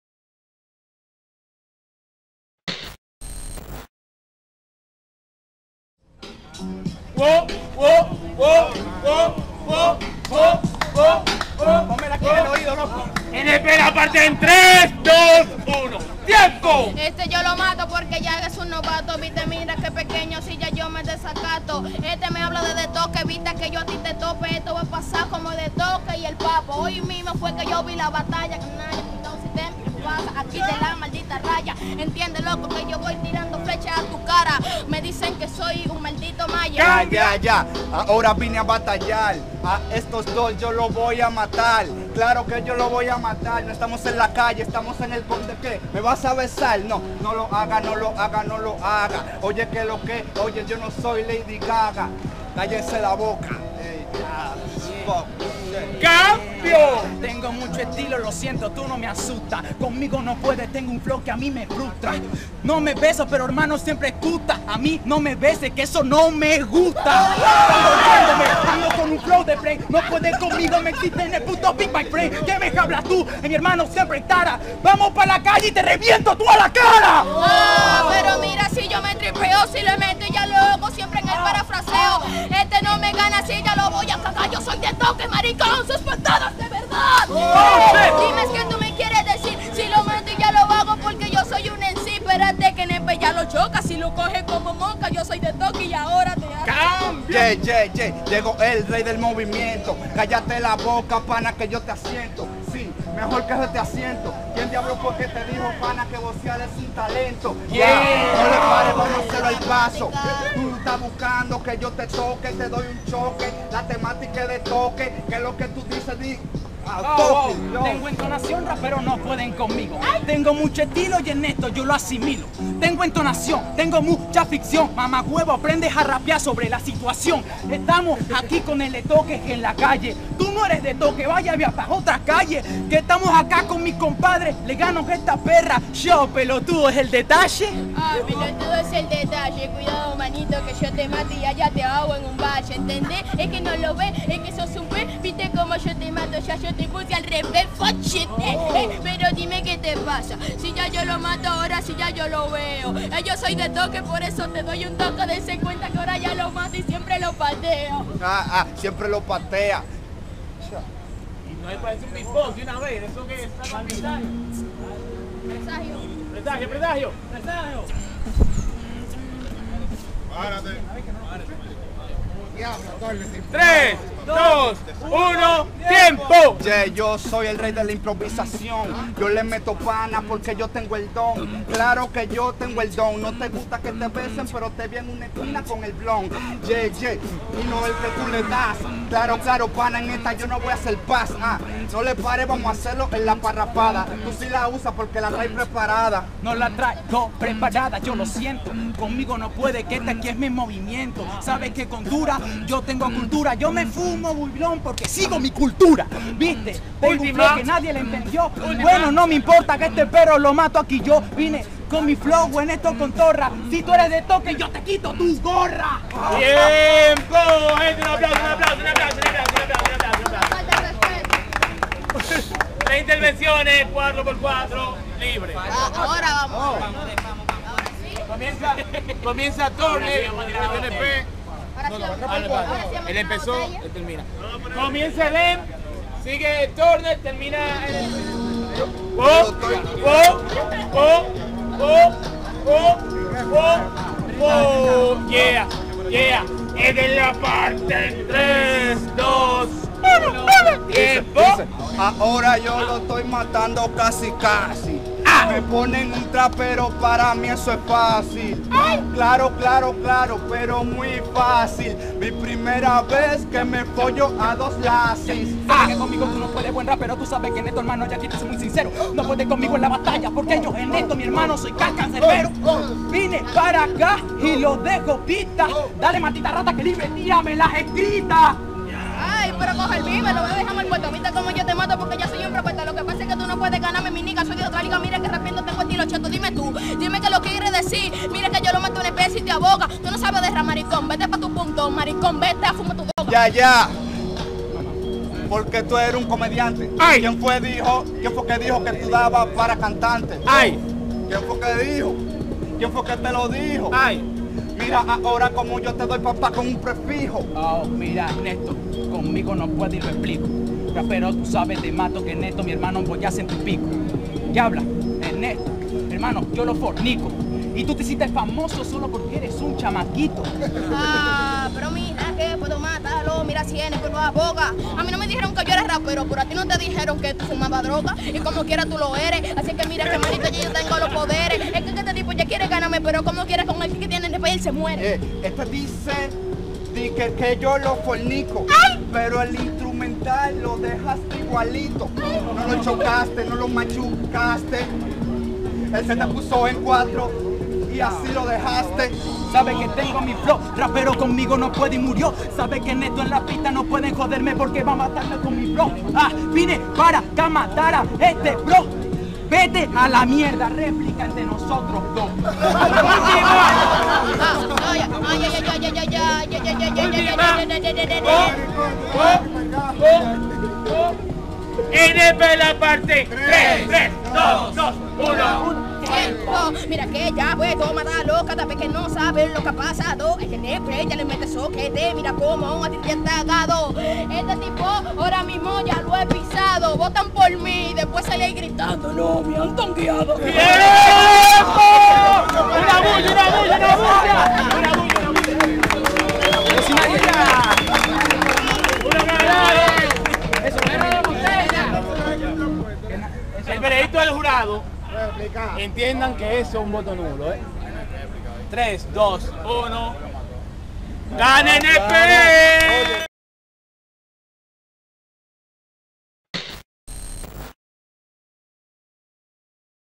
NP vs Lil Turner vs Eden | Duelo de Maestros (VIII Edición) 4tos.
Esta batalla sucedió en la octava edición de Duelo de Maestros entre los Mc´s NP, Lil Turner y Eden Revolución.

Cualquier critica o duda, déjala en la caja de los comentarios.